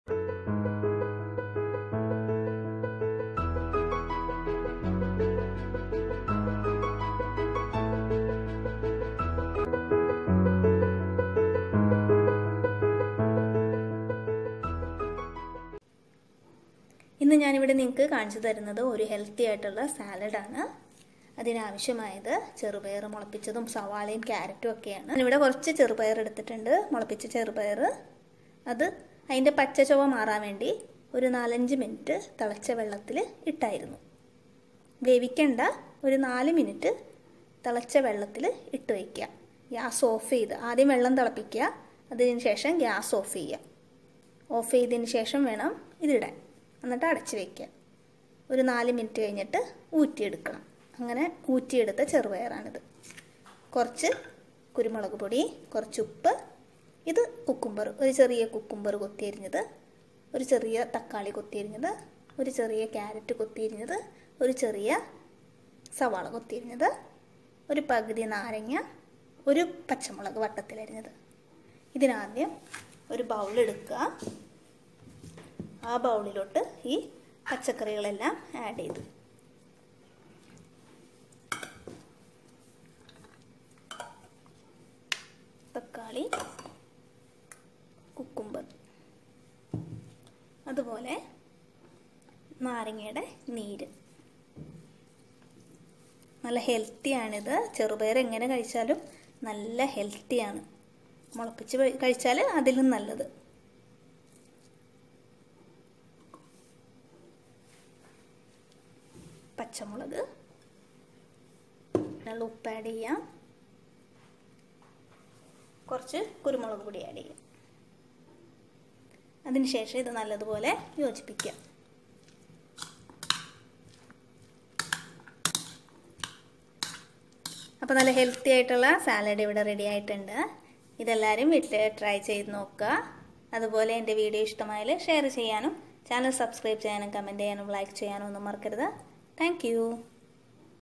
Healthy salad / sprouted green gram salad / vegetable salad / cucumber salad
ingredients

sprouted green gram
carrot
onion
tomoto
cucumber
green chilli
pepper powder
lemon
salt